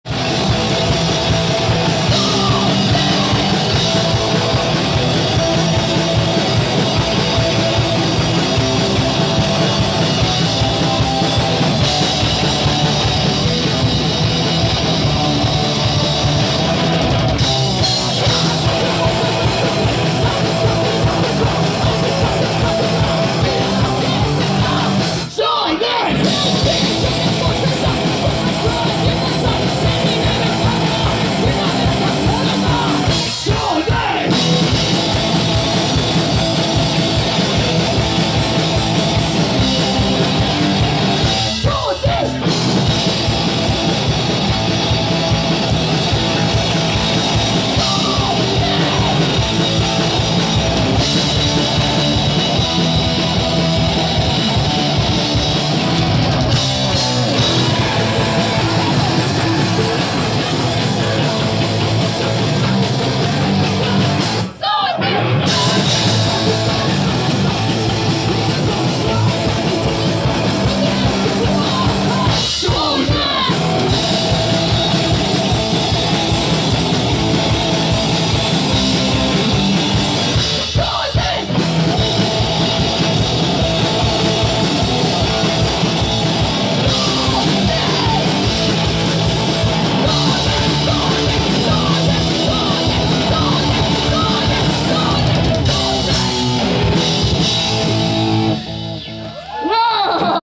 Pestpocken Refuse 2018 Ujz Peine 28 7 2018
Pestpocken
live auf dem Refuse 2018 Ujz Peine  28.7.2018
all pictures are there